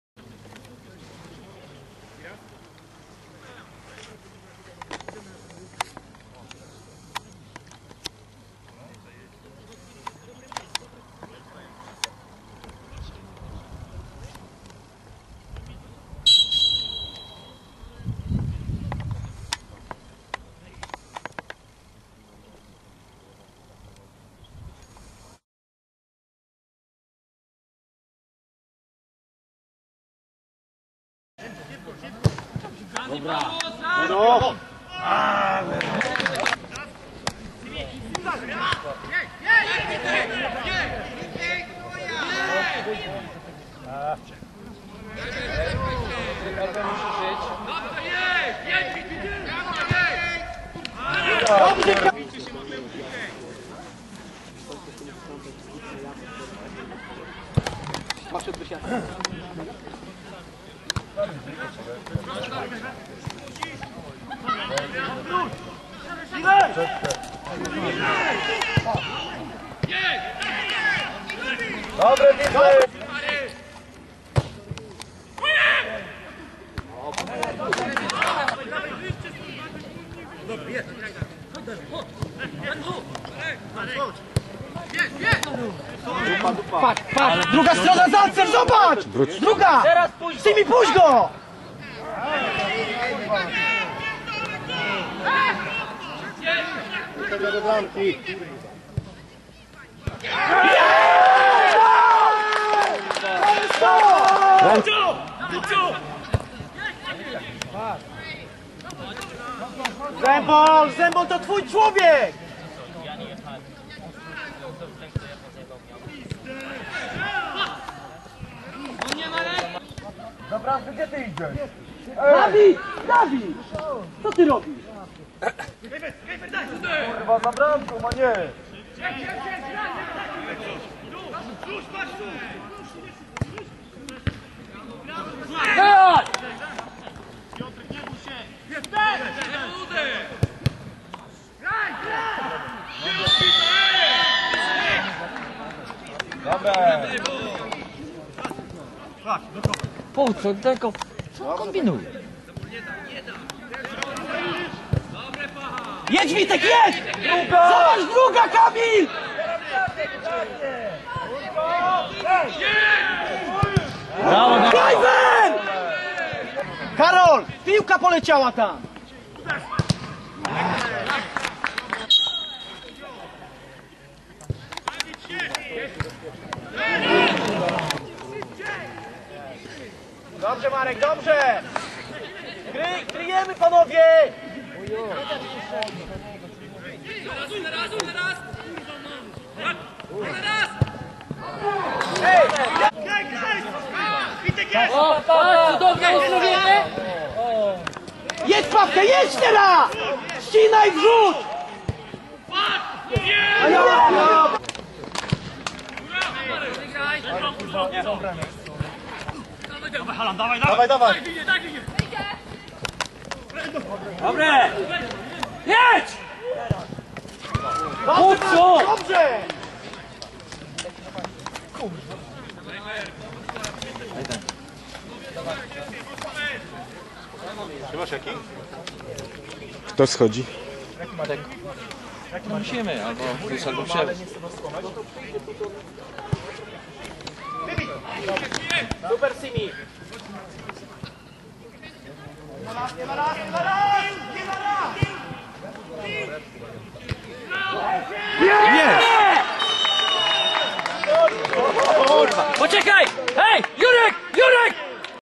Polonia Giżyn Wicher Przelewice 13.10.2013
Polonia Giżyn - Wicher Przelewice
1 - 1
1:0 - 44' -- K. Witkowski
1:1 - 75' -- P. Bil as. P. Antkiewicz

10  kolejka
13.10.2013
Reg. Klasa Okręgowa - gr. Szczecin Płd. (2013/14)

Minutą ciszy uczczono pamięć tragicznie zmarłego  Bartka Kadłubka (lat 16) - zawodnika LKS Wicher Przelewice.